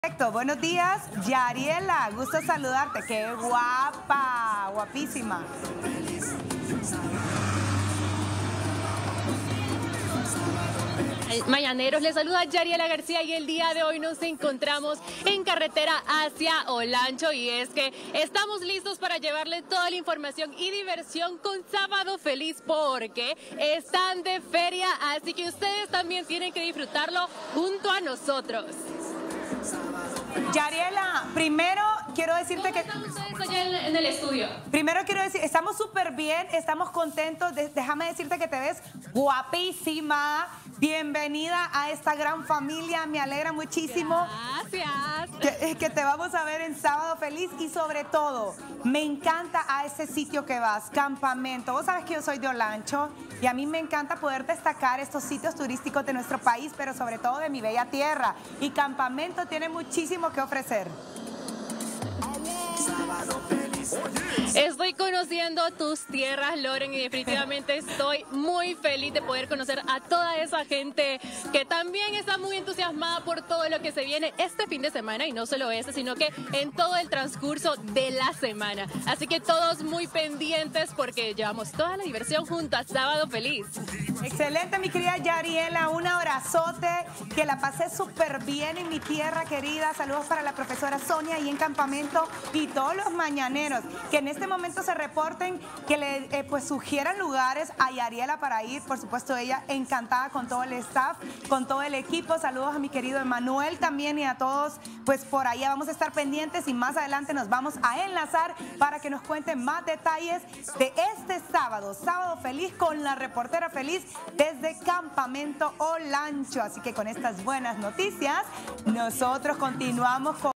😍 Yariela García, la reportera Feliz que deslumbró LM5
Bievenida Yariela Garcia que se dirige a Campamento, Olancho y se suma a la familia de TVC.